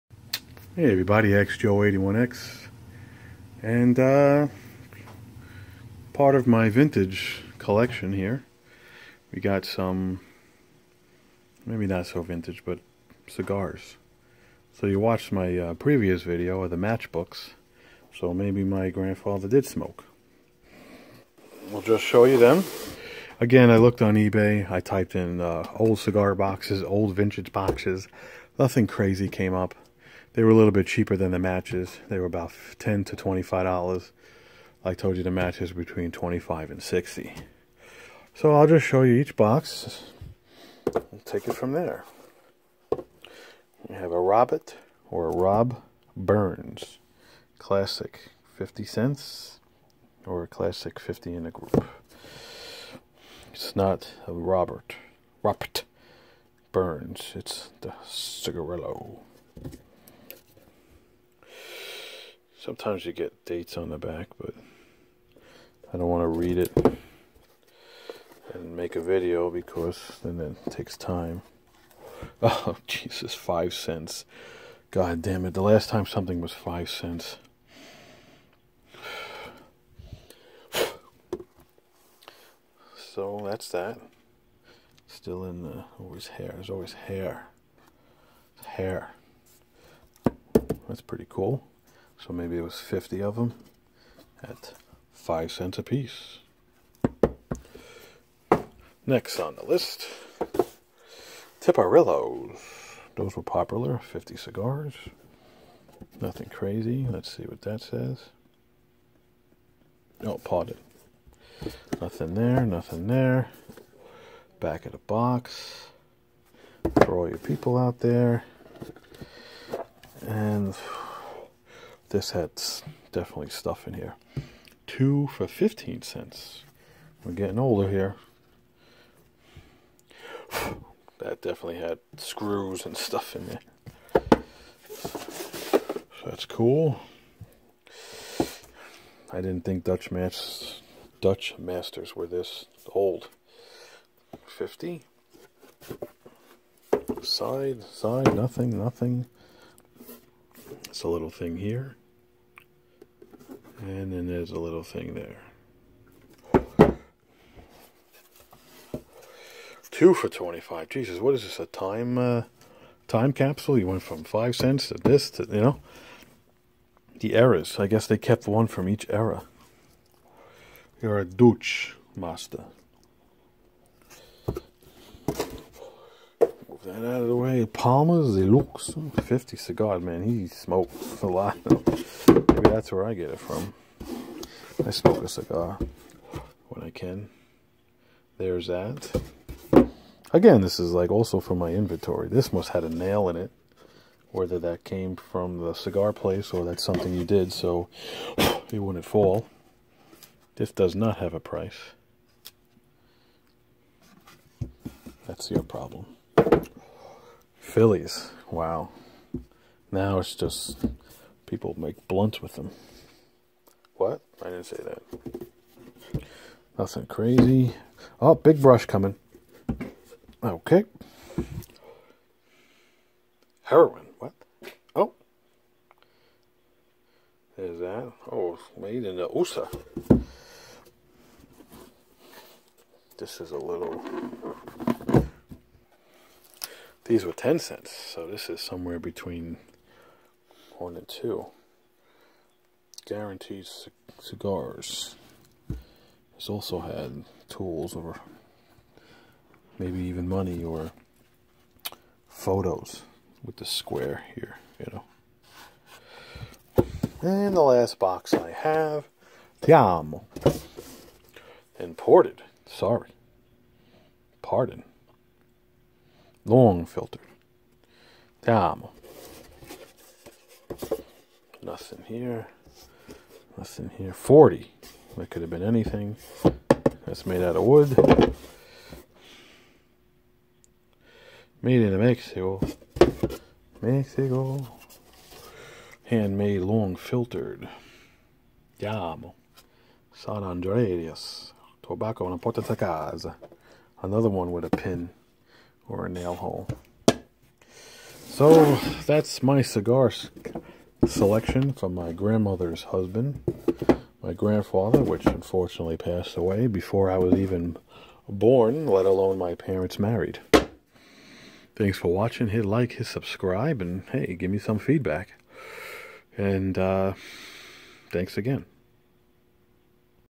Vintage Cigar Boxes
My grandfather must have smoked. Everyone smoked back in the day. Then the cigar boxes were a great way to store stuff.